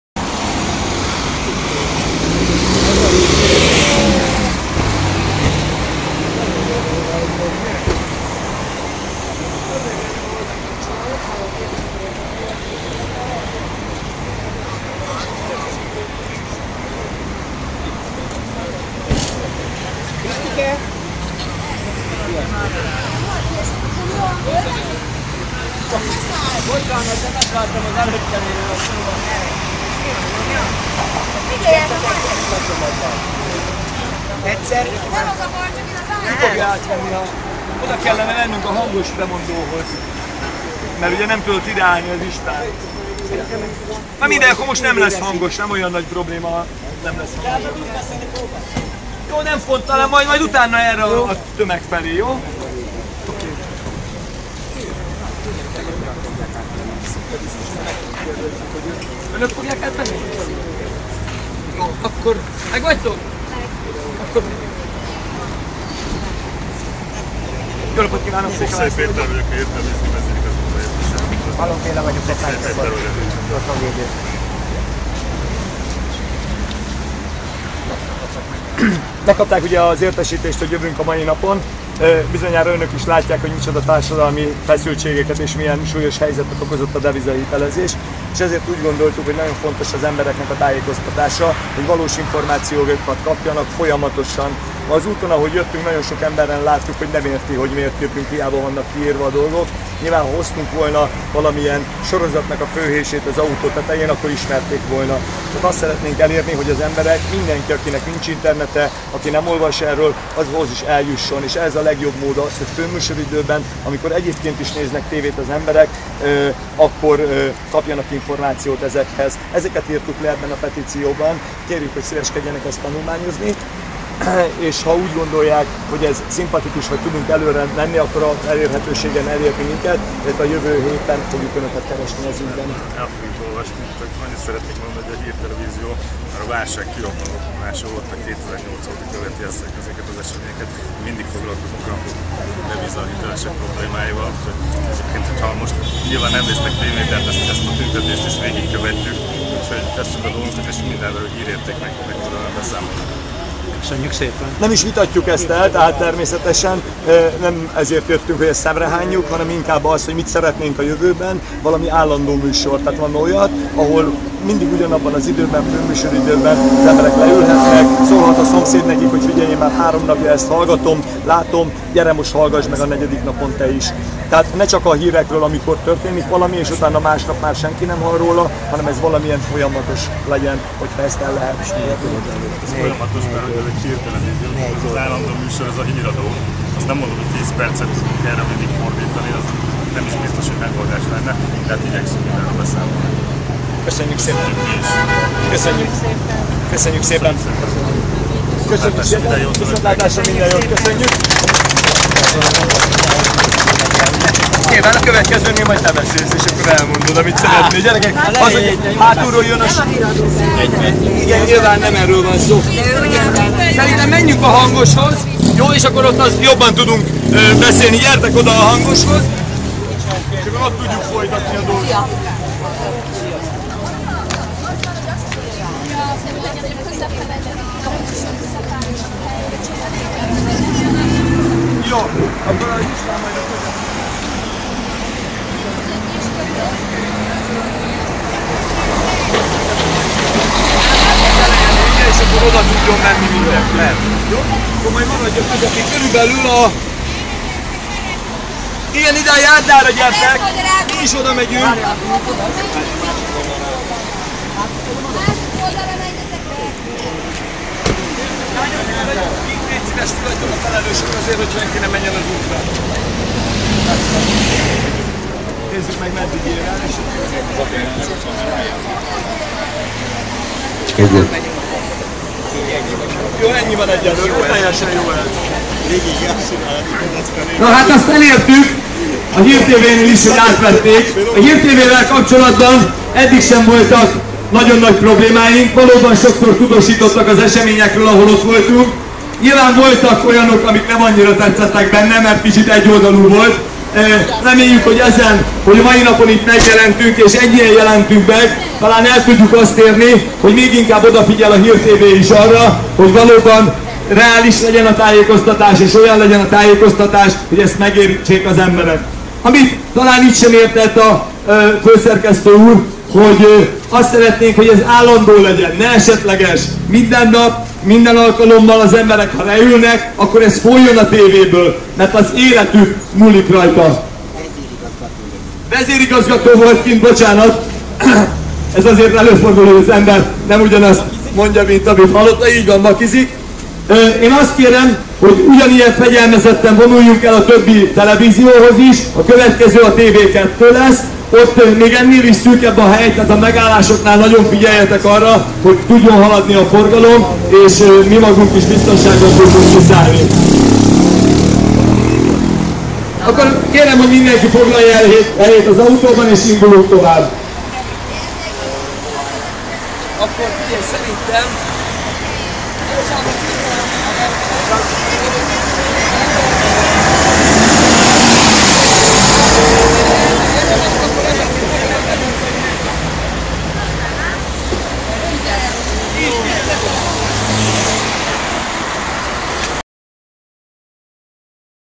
7/4 2013.07.19 AUTÓS VONULÁS BUDAPEST (HÍRTV)
Szikszai Péter vezérigazgatója vette át a petíciónkat, köszönjük, hogy megtisztelt minket. Tudjuk, hogy a HírTv minden demonstrációnkon részt vesz, közvetíti az eseményeket, foglalkozik a deviza-károsultak ügyével, tőle -a vezérigazgató úrtól- annyit kértünk, hogy hallgattassék meg a másik fél is, a civil szervezetek képviselői által, 10 perces műsoridőre nem kaptunk ígéretet, de a petíció áttanulmányozása után ígéretet kaptunk, hogy visszatérhetünk a párbeszédhez.